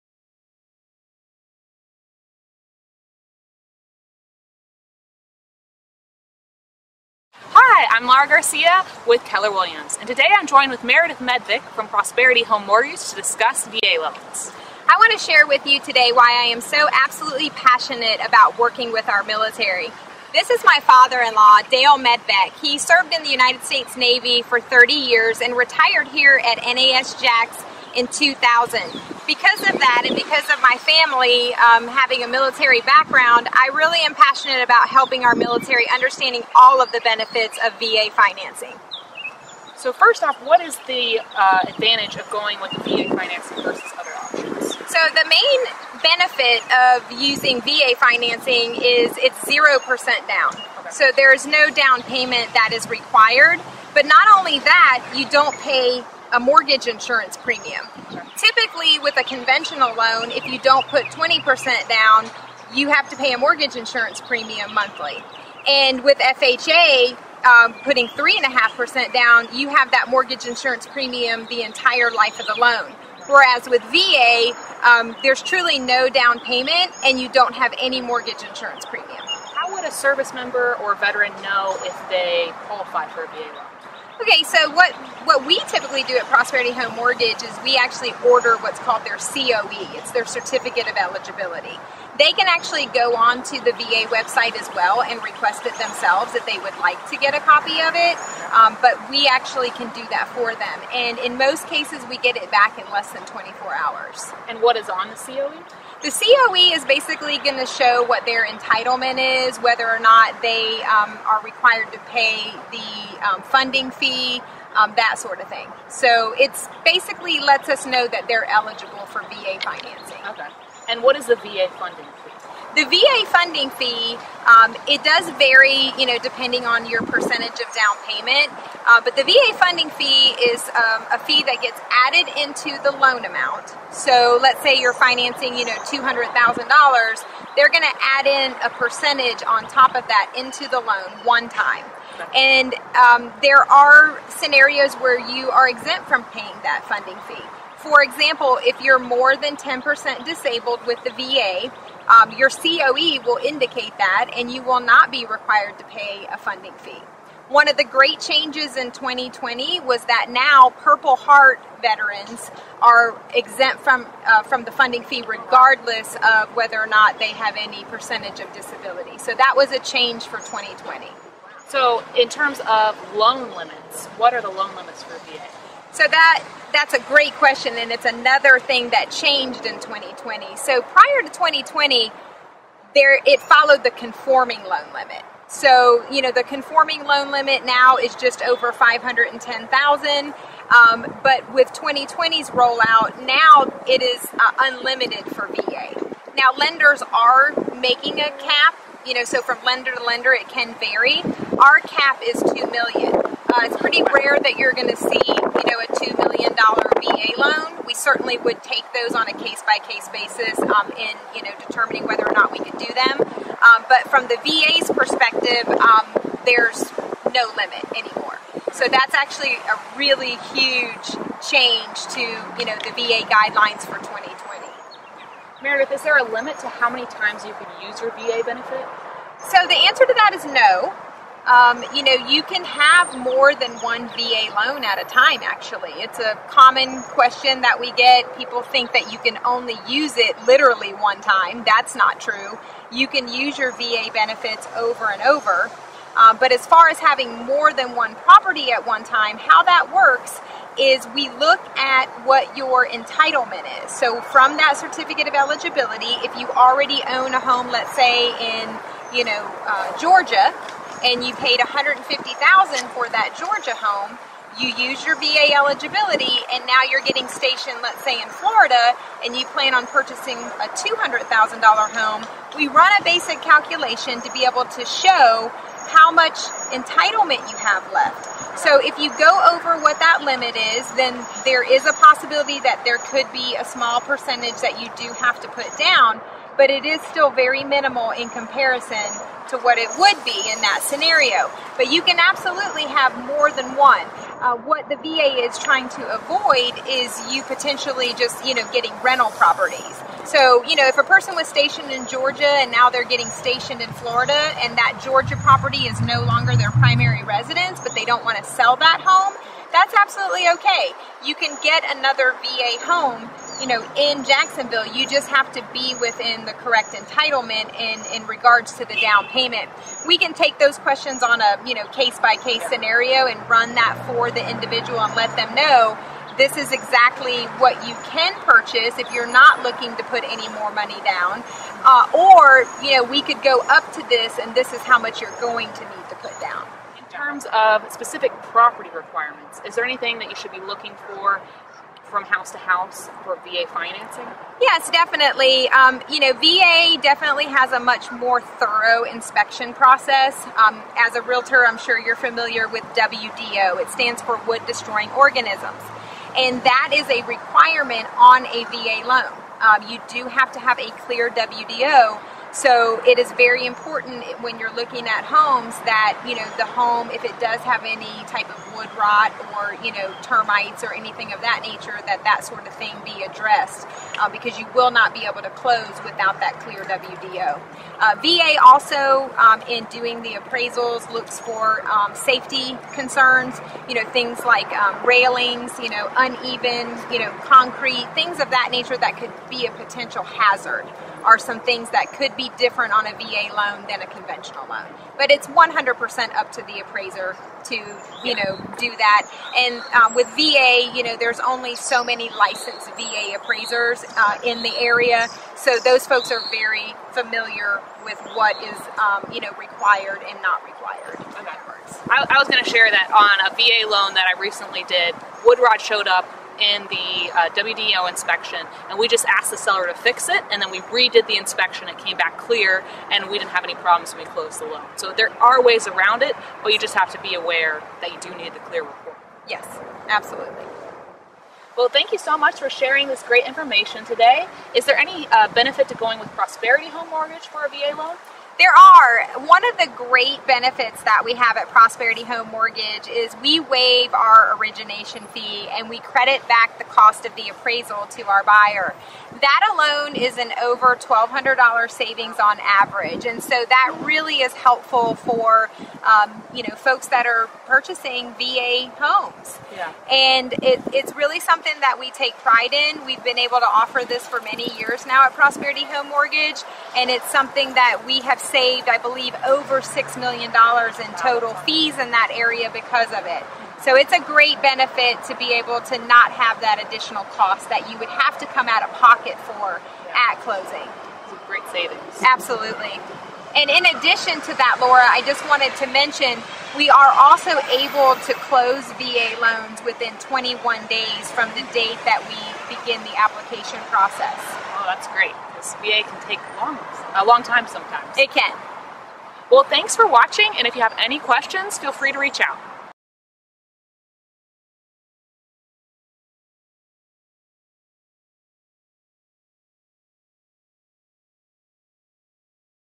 VA Financing Explained
A huge thank you to our active military and veterans! We would not enjoy the freedoms we have in this country without your service.  In honor of your sacrifice, we created a VA Financing video dedicated to you!

Meredith Medvec with Prosperity Home Mortgage, LLC answers common questions about Veteran's Administration Loans, or VA Loans:
- What are Advantages to VA Financing?
- How Would a Service Member or Veteran Know if They Qualify?
- What is a VA Funding Fee?
- Are there any specific property requirements for VA Financing?
- How many times can a service member use my VA benefit?
- Is there a cap to a service member's VA benefit?
- What Benefits does Prosperity Home Mortgage Offer Veterans?